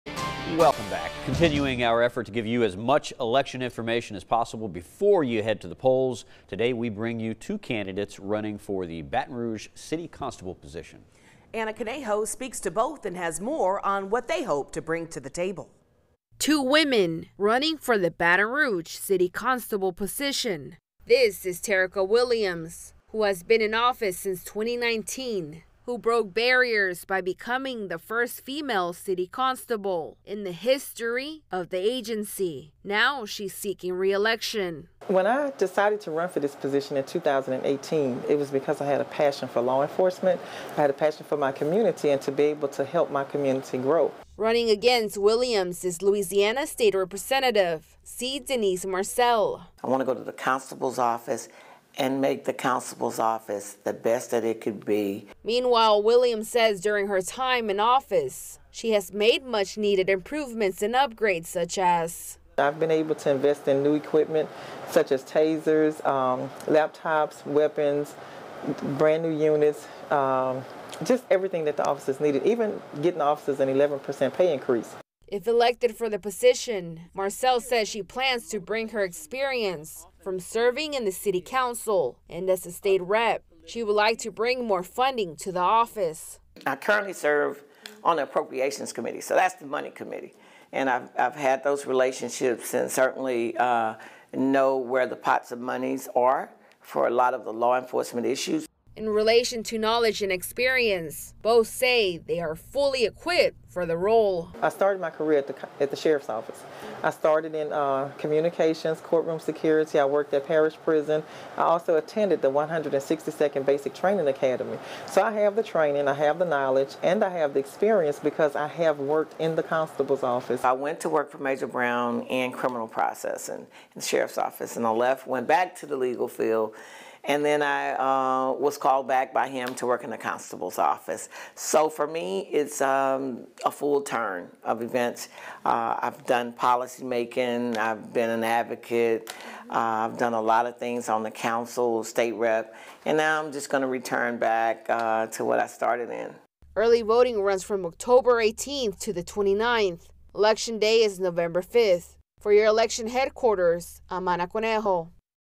Two well-known women battle for Baton Rouge City Constable position: hear their goals, plans
Current Baton Rouge Constable Terrica Williams is running for re-election against State Rep. Denise Marcelle. Both speak on why they want the position in office and what they bring to the table.